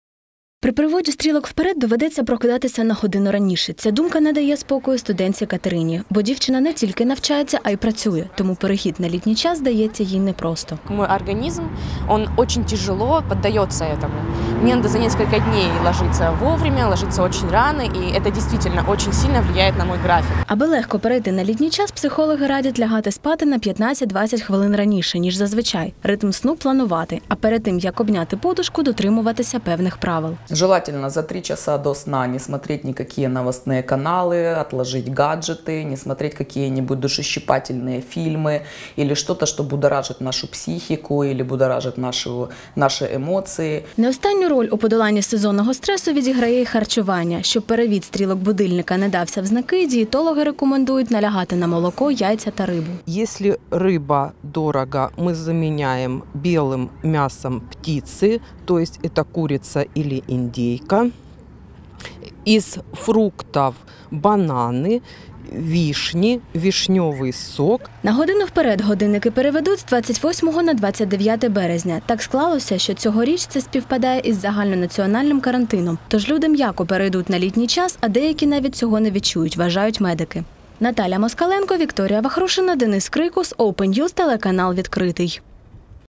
Як перейти на літній час правильно: поради психологів та медиків
Від зими – на літо. Уже цієї неділі українці переведуть стрілки будильників на годину вперед. А значить, прокидатися будемо раніше. Хоча перехід на літній час планували скасувати. Та все-таки не стане виключенням і ця, остання, неділя березня.